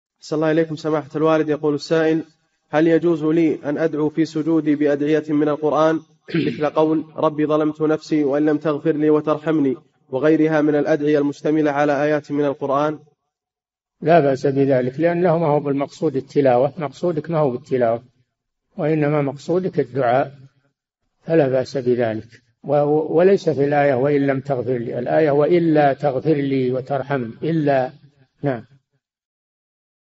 [736- 4717] حكم الدعاء في السجود بأدعية من القرآن - الشيخ صالح الفوزان
"[4717-736] هل يجوز لي أن أدعو في سجودي بأدعية من القرآن مثل قول (ربي ظلمت نفسي وإن لم تغفر لي وترحمني..) وغيرها من الأدعية المشتملة على آيات من القرآن؟
الجَوَابُ: لا بأس بذلك لأنه ما هو بالمقصود التلاوة مقصود، وإنما مقصودك الدعاء فلا بأس بذلك، وليس في الآية (وإن لم تغفر لي...) الآية ﴿وَإِلَّا تَغْفِرْ لِي وَتَرْحَمْنِي﴾ [هود: 47] وإلا.
فتاوى المجالس في تفسير المفصل
للاستفسار والإقتراحات والدعم
رئيس الفريق
لمشروع كبار العلماء